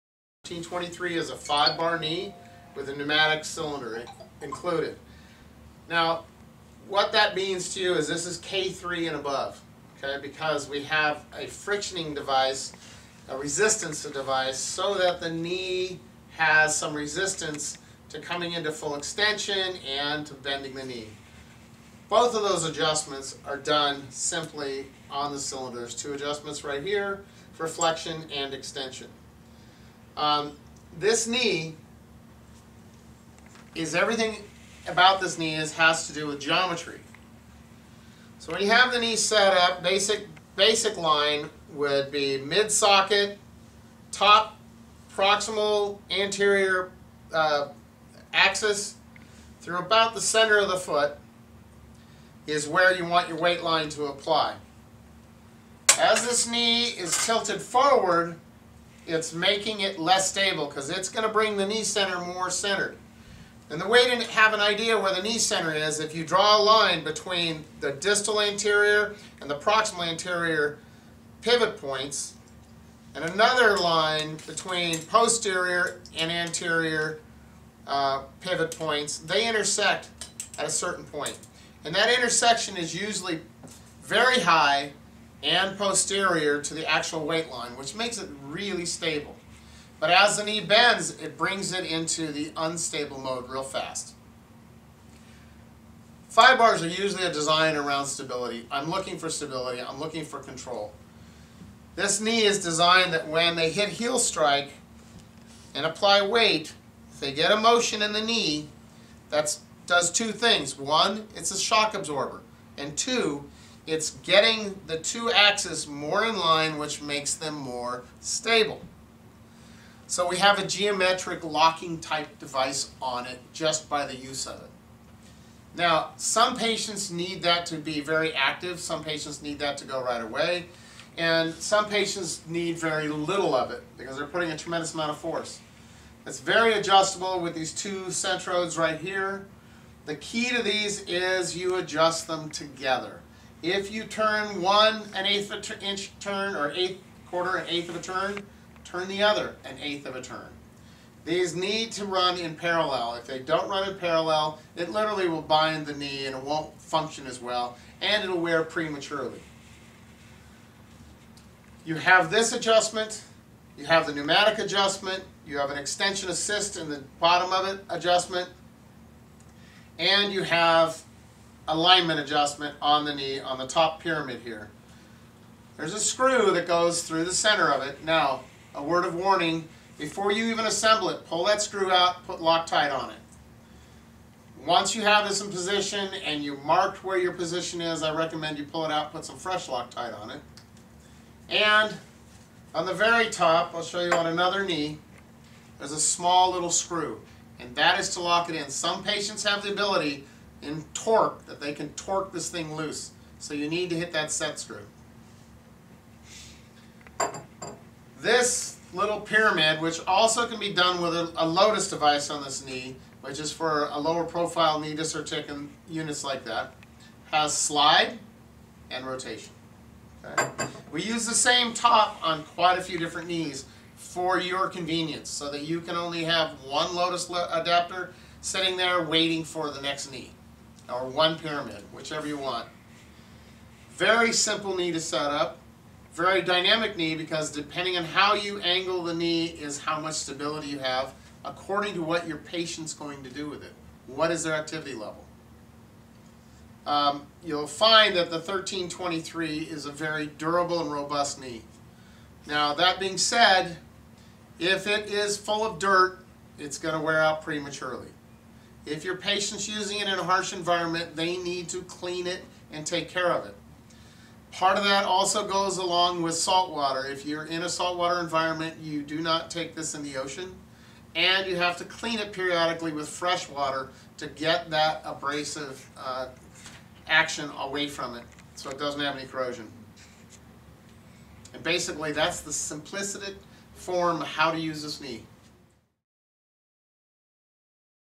5 bar Polycentric Pneumatic Kneejoint with stance flexion system (S600)
- Very stable 5 Bar Knee with a large stance flexion angle (up to 12°)
- Pneumatic cylinder with independent flexion extension  adjustment for swing phase
- Can be used for either above knee or knee disarticulation
- All axes have built-in bearing which allow for ultra smooth walking movement